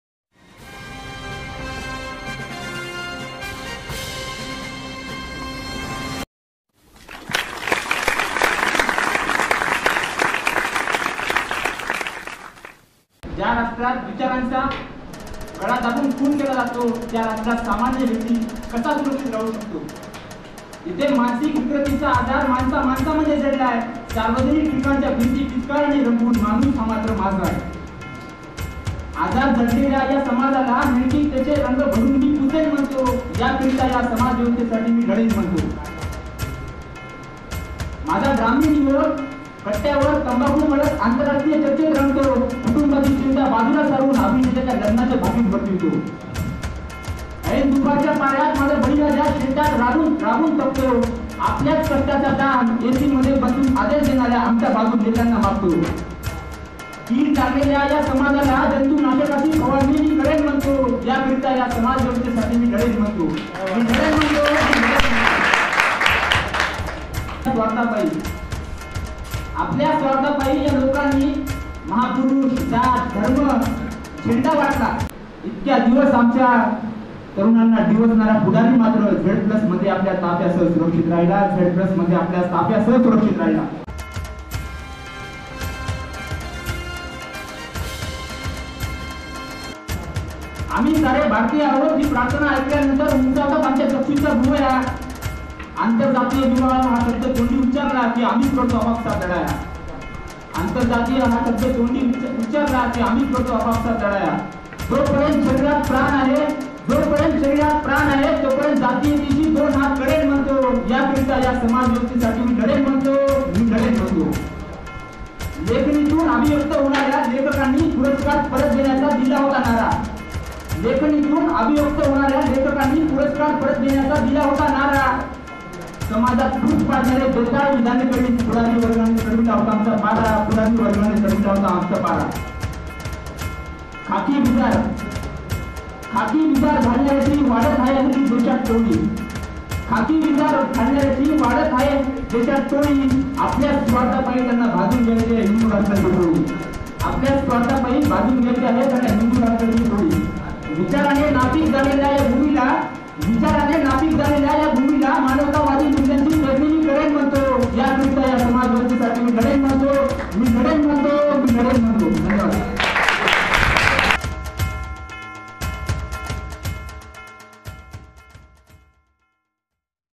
लढेन म्हणतो ...[समाजाच्या संध्यपरिस्थितीवर भाष्य करणारी स्व:रचित विद्रोही कविता:by Pratik kukade]
At conference hall department of mass communication and journalism dr.babasaheb ambedkar marathwada university ,aurangabad
edit : PMO production...